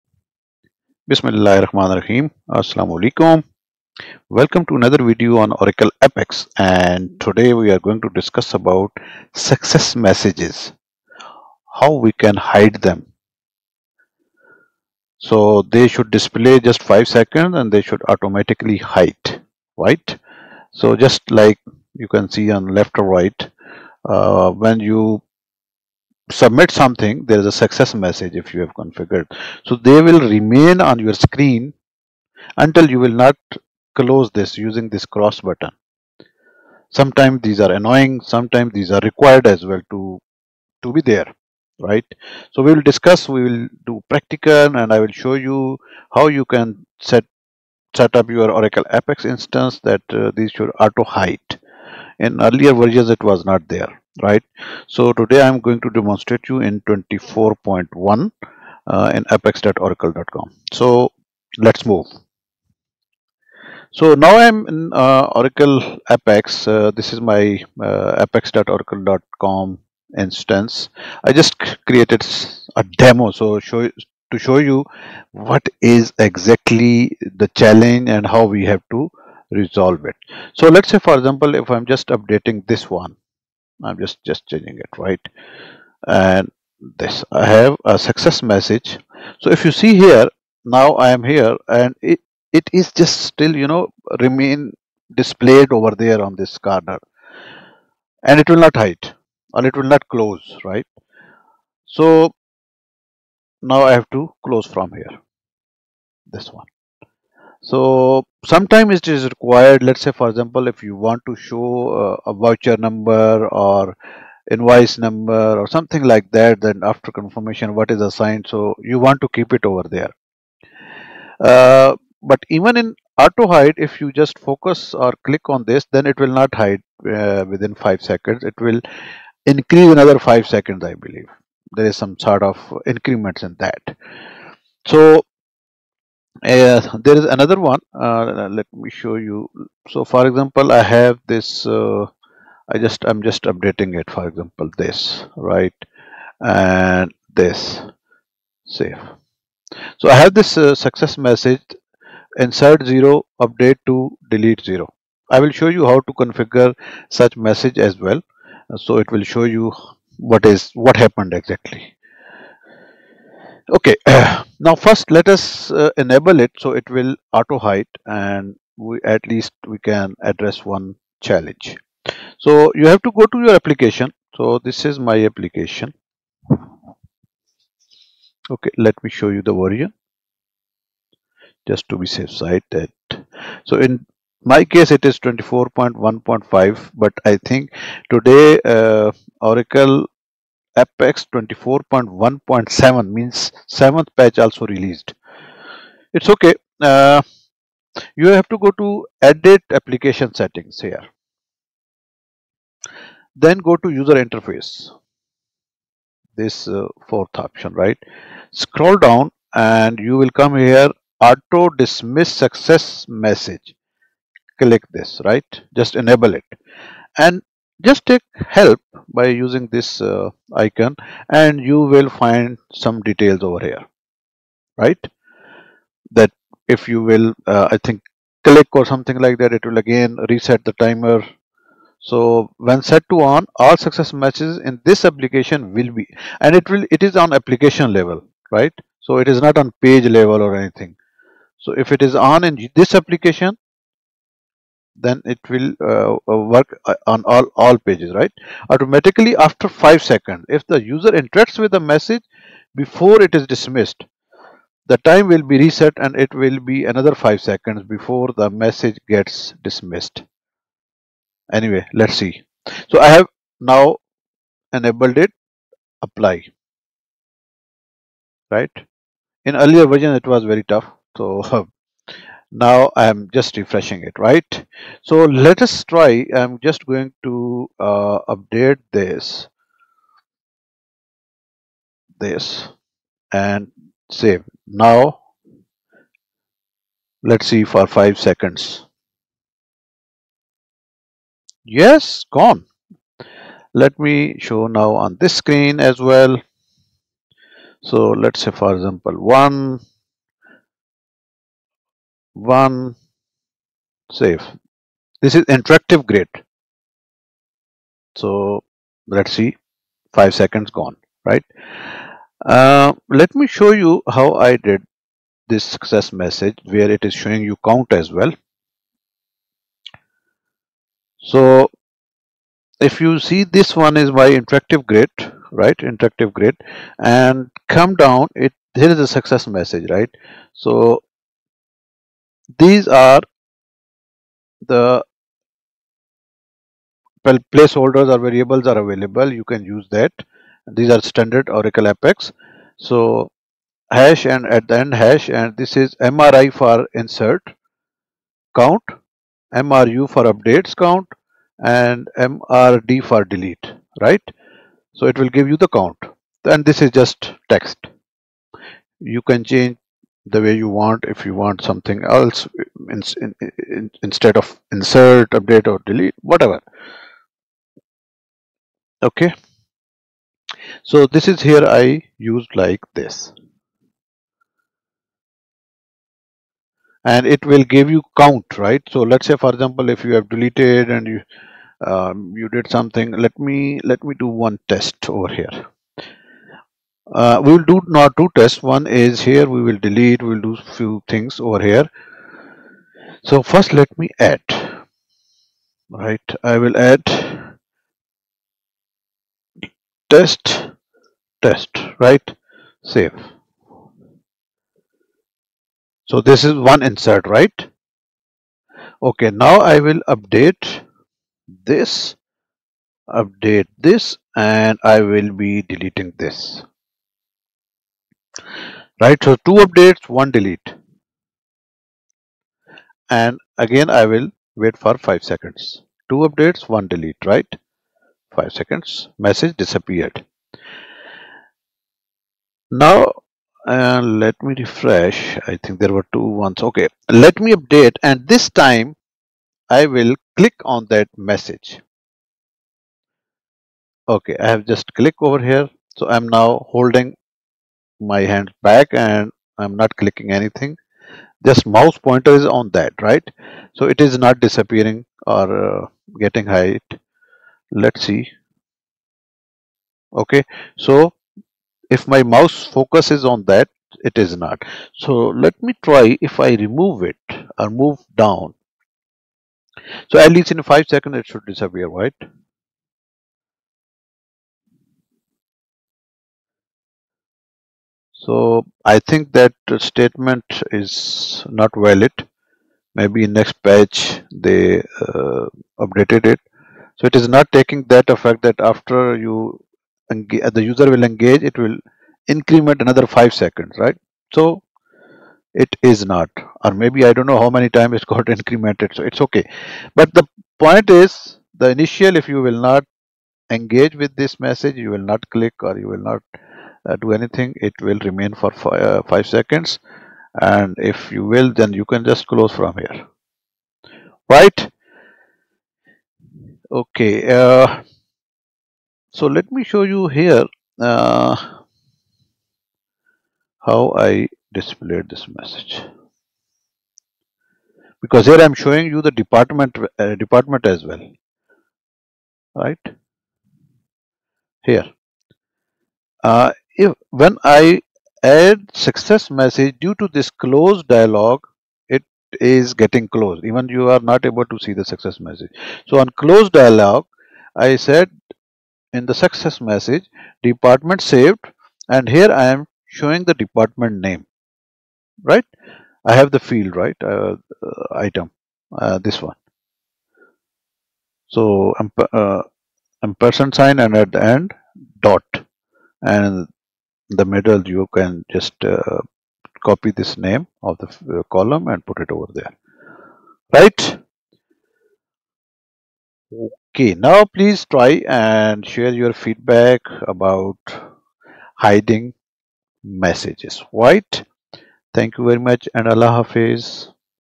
How to Customize Success Messages in Oracle APEX 24.1: Auto-Hide After 5 Seconds
In this video, I explore the concept of Success Messages in Oracle APEX, focusing on how these notifications enhance the user experience by providing instant feedback. I demonstrate the process of configuring these messages to effectively convey the success of actions performed within an application. You'll learn the step-by-step setup to enable and customize success messages, ensuring they align with your application's design and functionality. This walkthrough is particularly tailored for Oracle APEX 24.1, highlighting the platform's updated features and capabilities in managing user notifications seamlessly.

Furthermore, I show how to set up success messages to automatically hide after 5 seconds, adding a dynamic touch to your application's interface. This setup is crucial for maintaining a clean and non-intrusive UI, preventing messages from lingering unnecessarily. By the end of the video, you'll have a clear understanding of configuring and fine-tuning success messages, making your Oracle APEX applications more user-friendly and professional. This tutorial is perfect for developers aiming to enhance their APEX applications with polished feedback mechanisms.